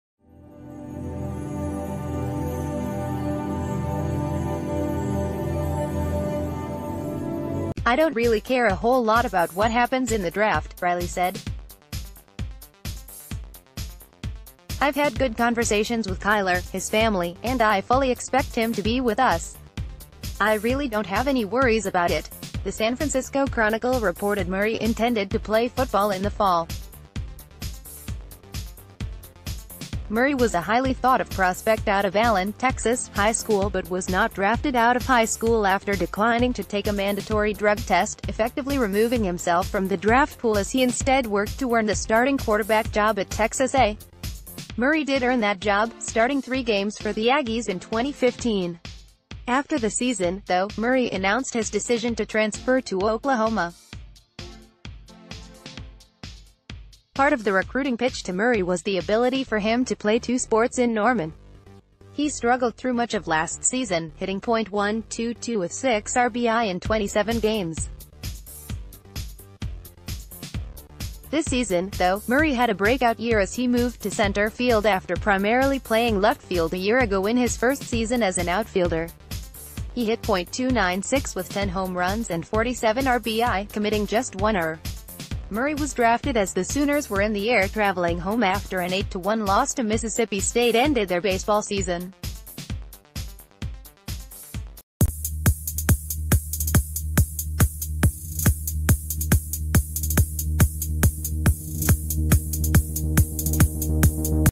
Breaking News | Sooners' Kyler Murray picked No. 9 by A's in MLB Draft, likely to continue football
Breaking News | Sooners' Kyler Murray picked No. 9 by A's in MLB Draft, likely to continue football career

“I don't really care a whole lot about what happens in the draft,” Riley said. “I've had good conversations with Kyler, his family, and I fully expect him to be with us. I really don't have any worries about it.” The San Francisco Chronicle reported Murray intended to play football in the fall. Murray was a highly thought of prospect out of Allen (Texas) High School but was not drafted out of high school after declining to take a mandatory drug test — effectively removing himself from the draft ...